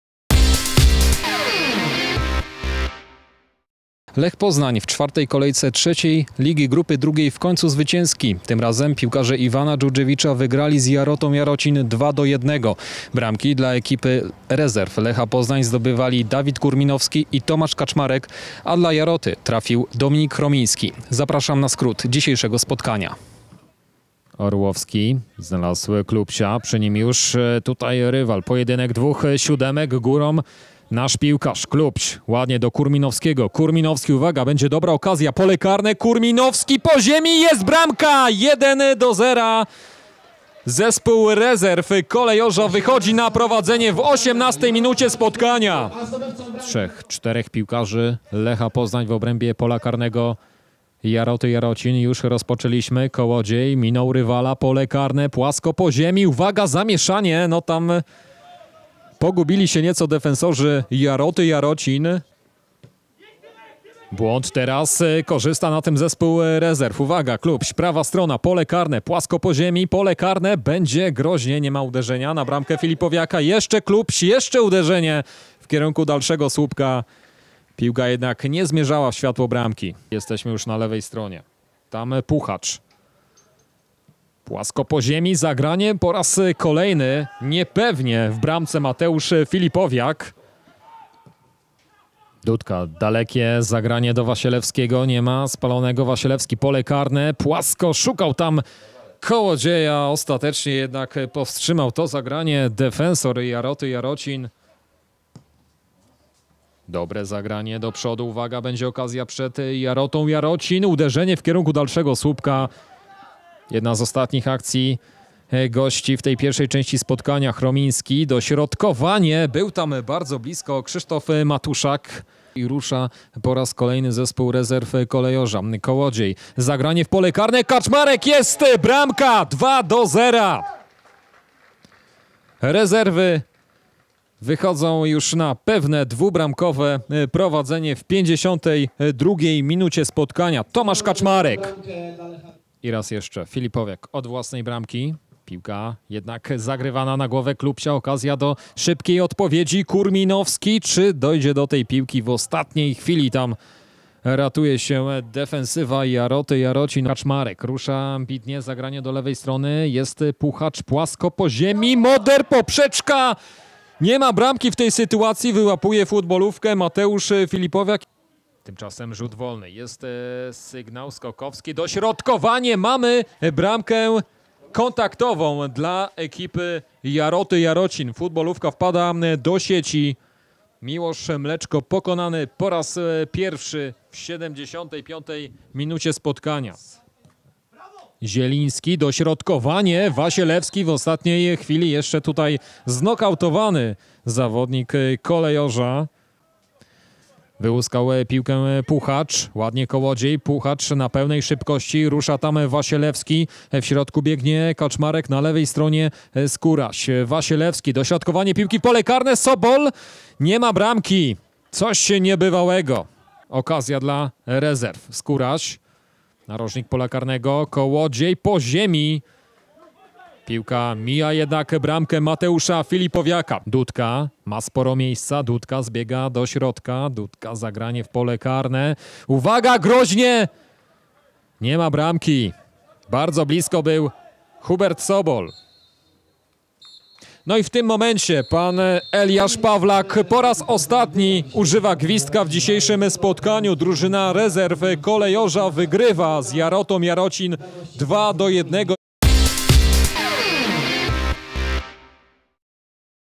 Lech II Poznań - Jarota Jarocin 2-1 (skrót meczu)
Rezerwy Kolejorza wygrały w sobotę pierwszy ligowy mecz w obecnym sezonie. Bramki dla lechitów zdobywali Dawid Kurminowski i Tomasz Kaczmarek.